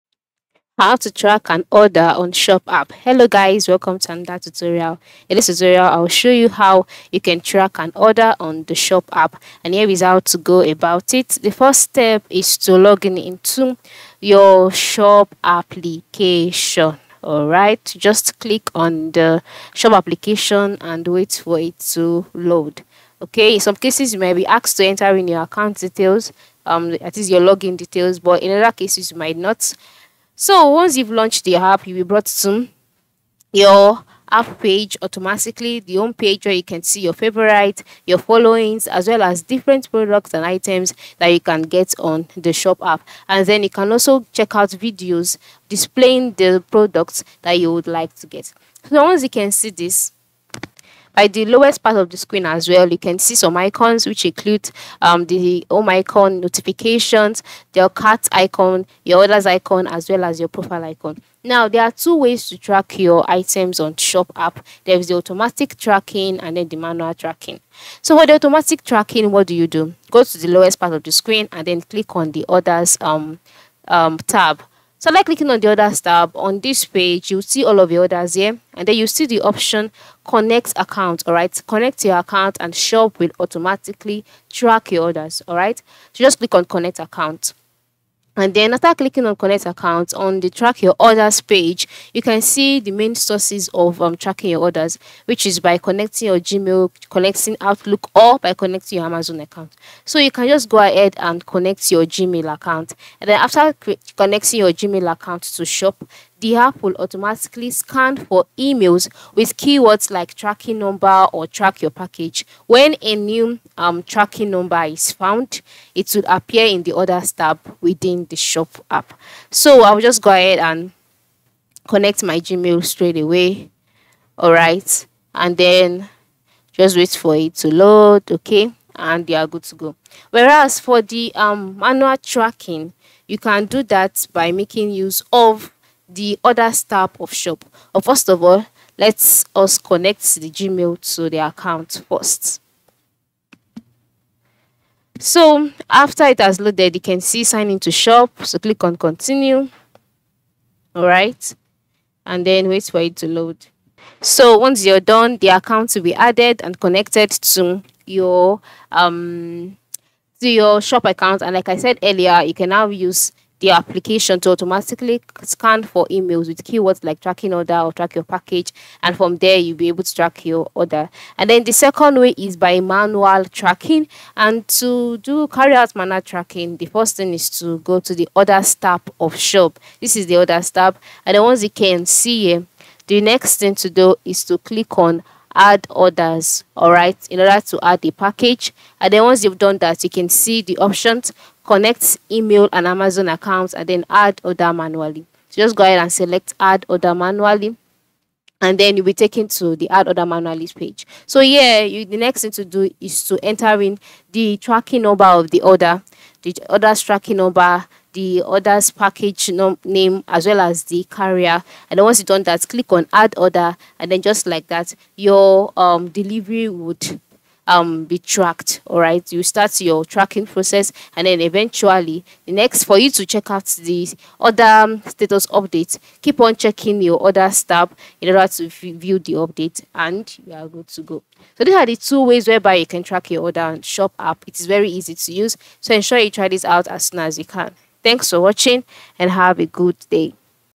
How To Track Order on Shop App (Quick Tutorial)
In this video I will show you How To Track Order on Shop App. It would be good if you watch the video until the end so that you don't miss important steps.